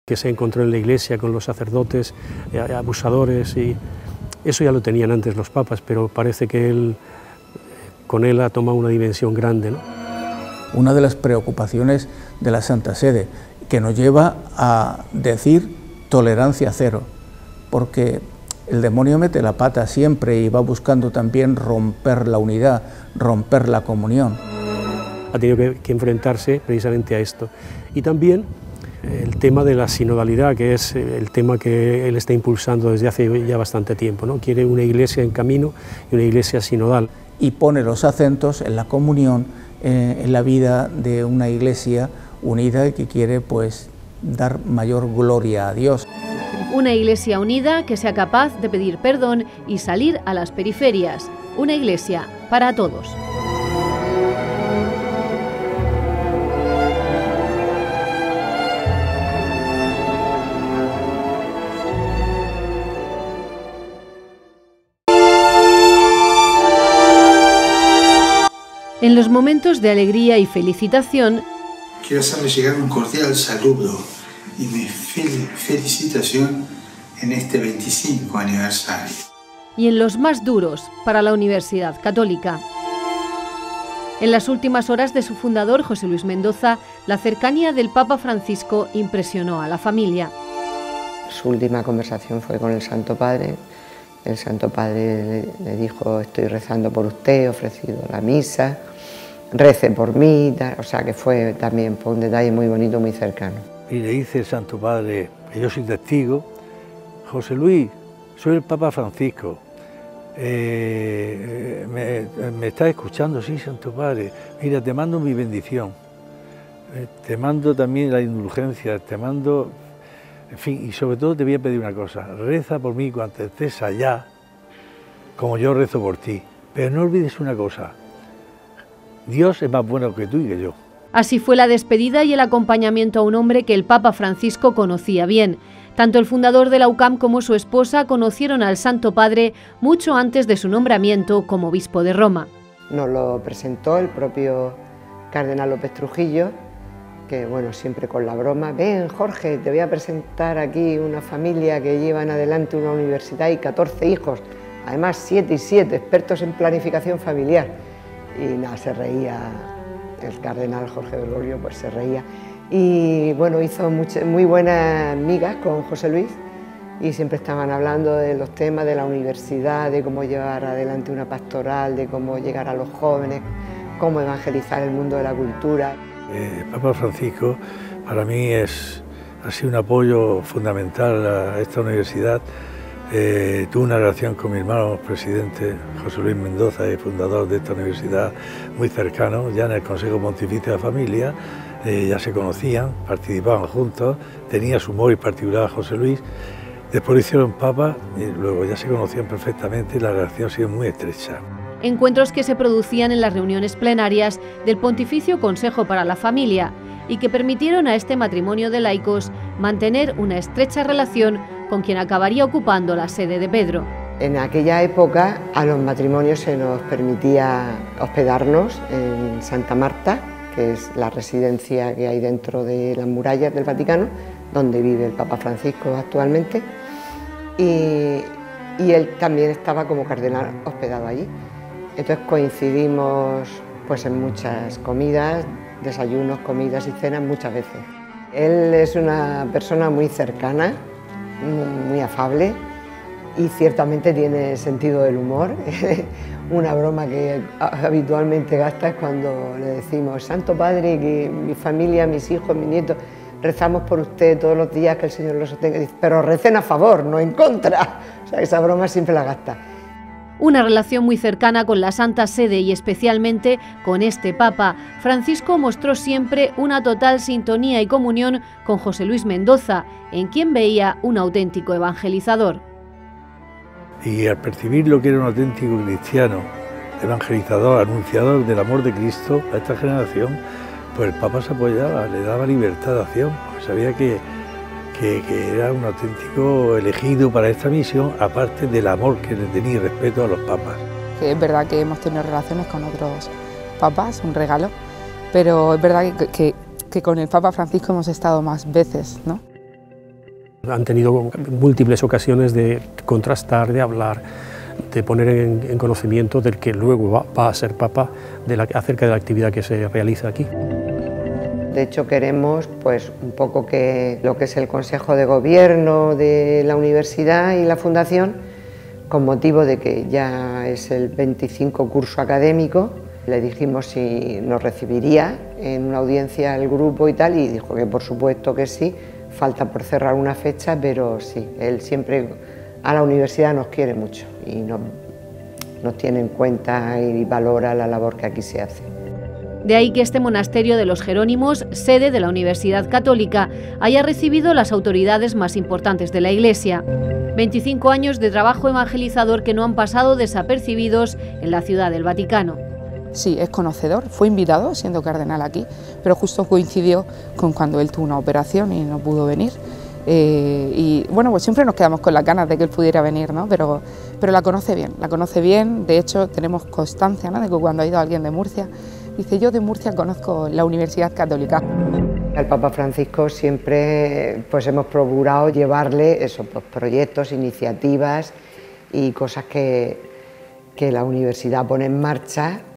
El Papa Francisco y la UCAM
A los 10 años del pontificado del Papa Francisco recordamos algunos de los momentos de la relación entre el presidente fundador de la UCAM o su familia con el Papa Francisco